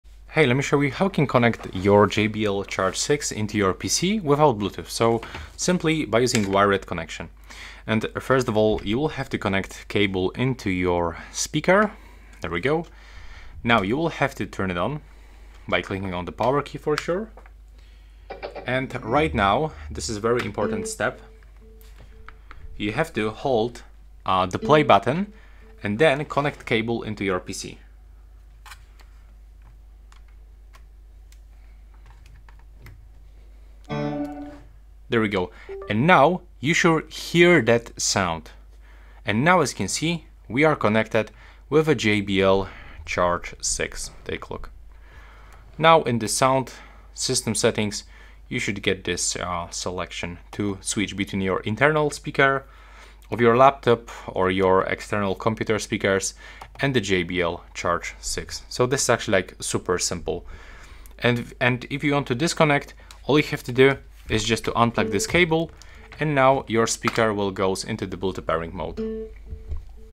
How to Connect JBL Charge 6 to your PC without Bluetooth!
In this video, I will show you how to use the JBL Charge 6 with your PC without a Bluetooth connection. You’ll learn how to connect the speaker using a wired setup, such as an AUX cable or USB-C if supported. I’ll guide you step by step through the process to ensure your JBL Charge 6 works seamlessly with your computer even without wireless pairing. This method is perfect for situations where Bluetooth isn’t available or stable.

I'll do my best to reply promptly and assist you.